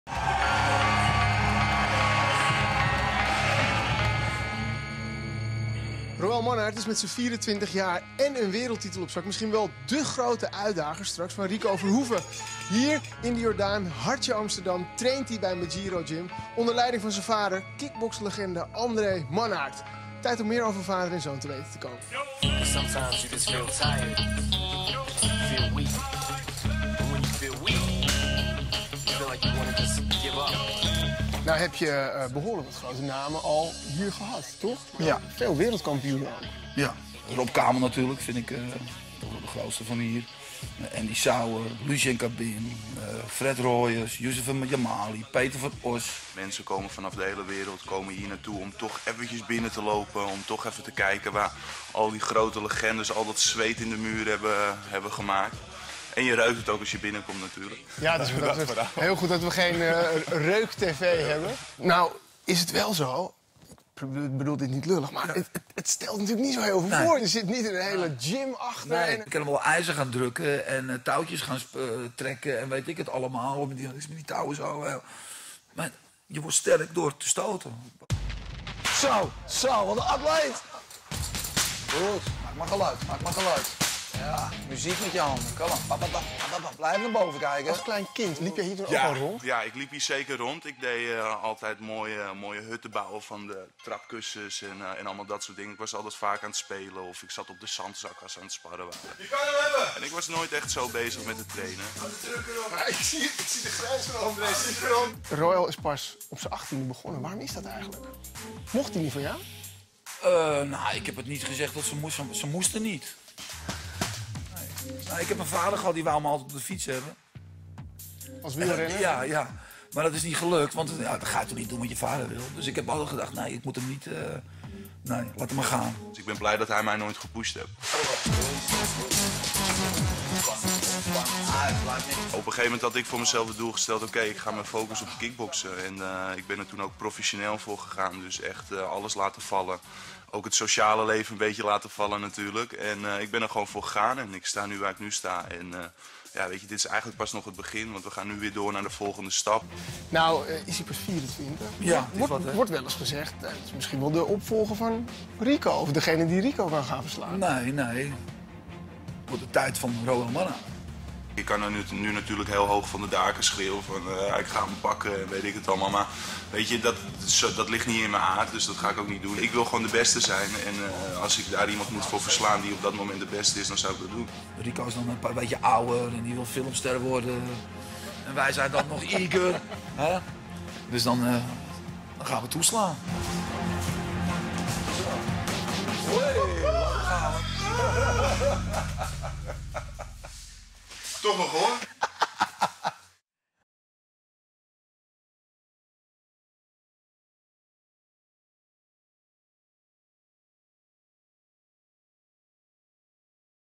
Is dit de opvolger van Rico Verhoeven? “Dit wordt de tijd van Roël Mannaart”
Roël Mannaart maakte afgelopen september in de Johan Cruijff Arena zijn GLORY-debuut. De zoon van kickbokslegende Andre Mannaart wordt als een van de mogelijke opvolgers van Rico Verhoeven als de GLORY-kampioen op zwaargewicht. Mark Schaaf ging langs bij vader en zoon in Amsterdam bij Mejiro Gym.

Over Veronica Sport NL
Welkom op het officiële YouTube-kanaal van Veronica Sport! Hier zie je de allerbeste actie en exclusieve clips van de UFC & GLORY Kickboxing!